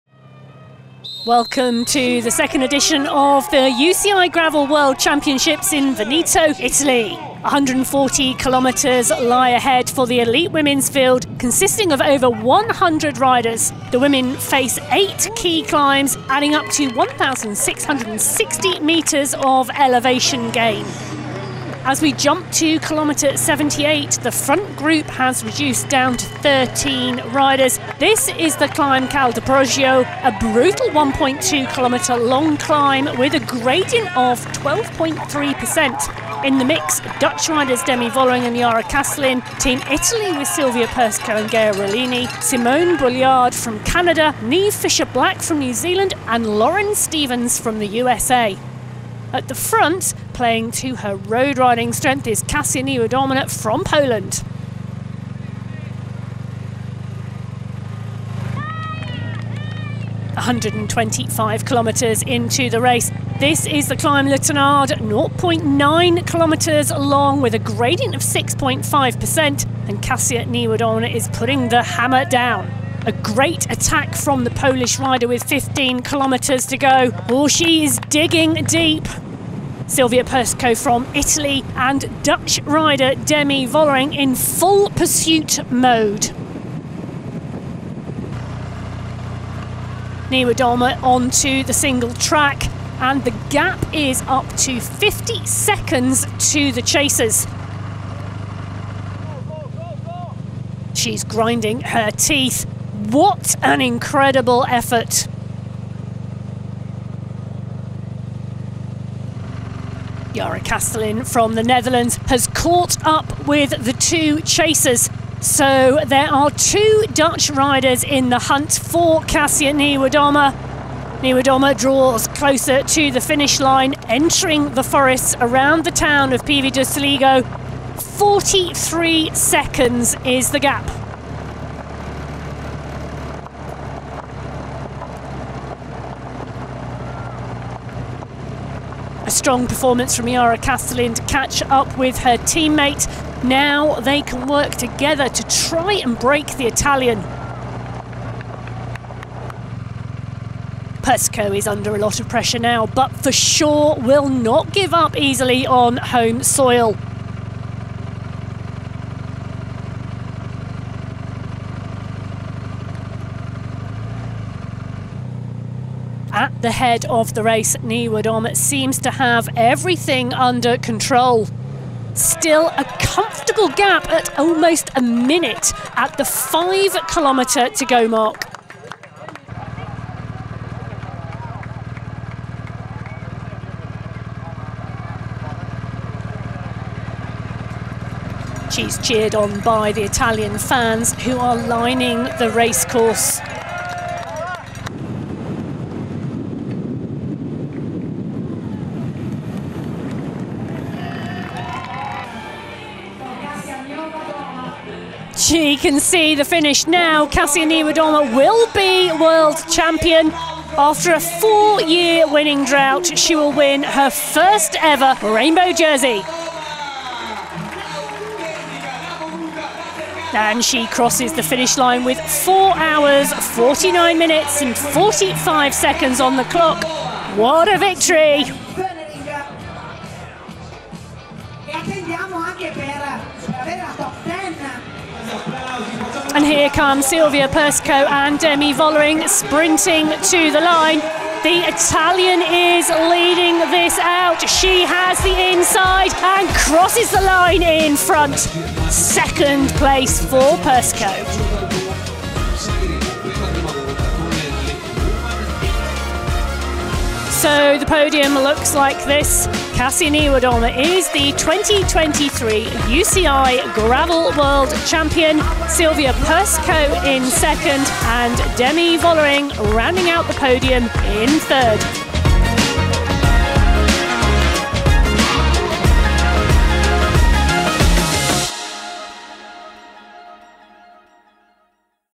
Women Elite Highlights | 2023 UCI Gravel World Championships
Watch the highlights from the Women Elite race at the 2023 UCI Gravel World Championships.

This was the second edition of the UCI Gravel World Championships, taking part in Veneto, Italy. The Women Elite were racing for the rainbow jersey over a course of 140km and 1660m of climbing.

Join the UCI's dedicated online platform, My World of Cycling